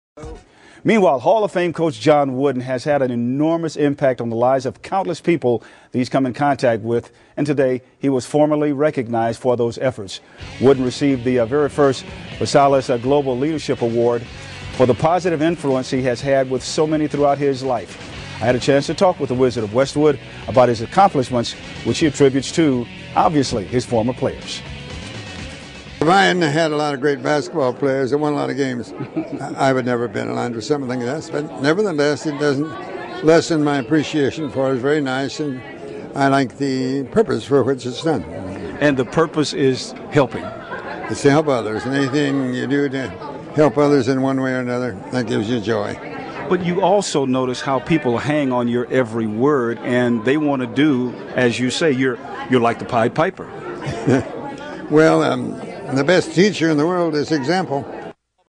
ViSalus - John Wooden receives Global Leadership Award
Hall of fame coach John Wooden received the first Global Leadership Award from ViSalus. Coach Wooden has been very influential to many individuals, changing many lives throughout the course of his career.